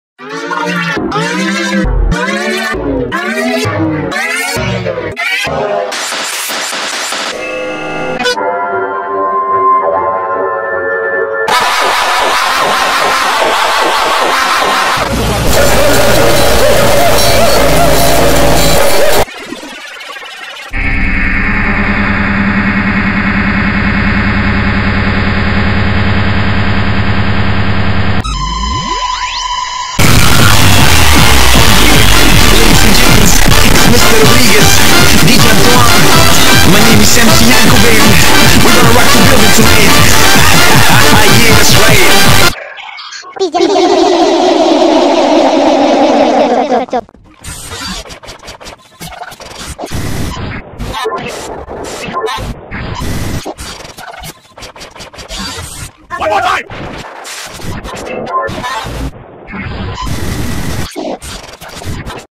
[21.04.2017] Second Rayman VideoPad Tennis Round 32 Beta
scrapped vegas pro version of round 32 vs myself (with special guest tbeg2006)